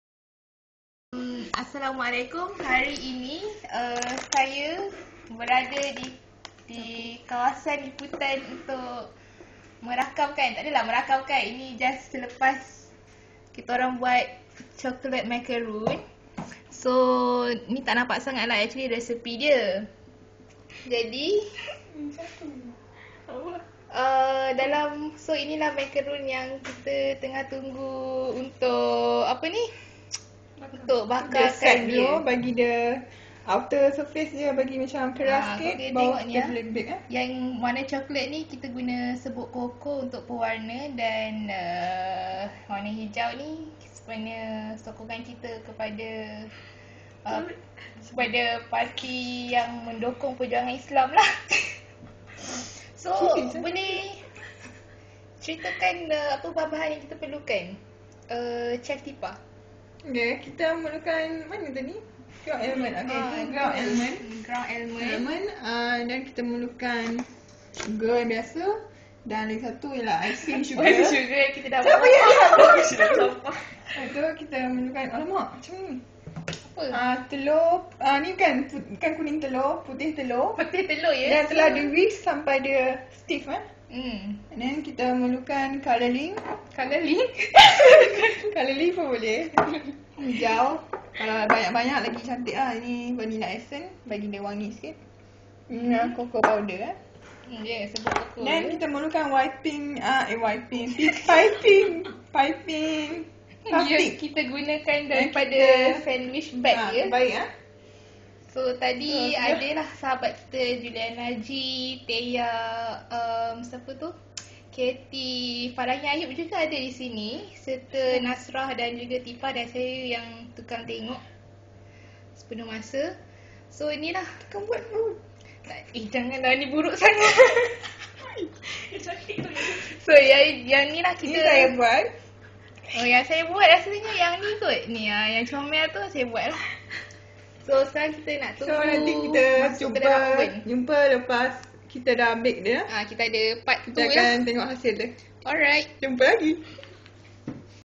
Macaroon Castlewhite 2012
Gadis castlewhite : teya, juliana jee, ketty, tipah, farahin, nasrah bersatu menghasilkan macaroon castlewhite.